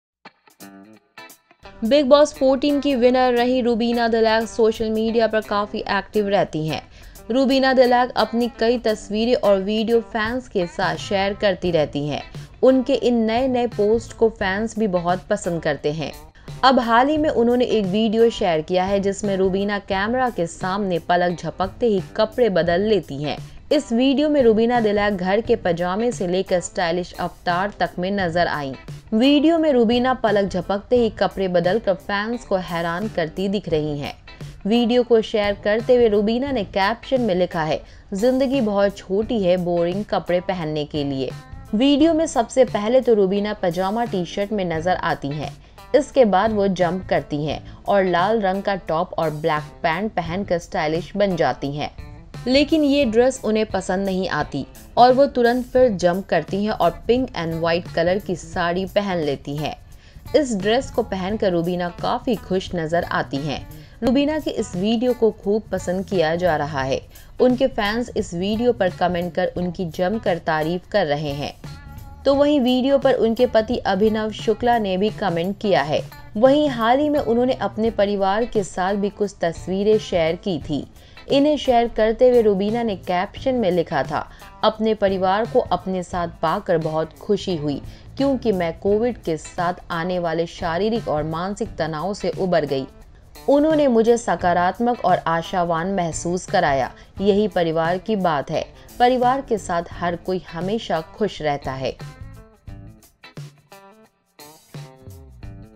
रुबीना दिलैक ने अलग स्टाइल में पहनी साड़ी, Photos देखकर लोग बोले- बॉस लेडी
Khatron Ke Khiladi Contestants: DivyankaTripathi​ , ArjunBijlani​ , AbhinavShukla​ ,VarunSood​ , AnushkaSen​ ,SanaMakbul​ ,MahekChahal​ ,NikkiTamboli​ ,RahulVaidya​ ,ShwetaTiwari​ ,SourabhRajJain​ ,VishalAdityaSingh​,AasthaGill​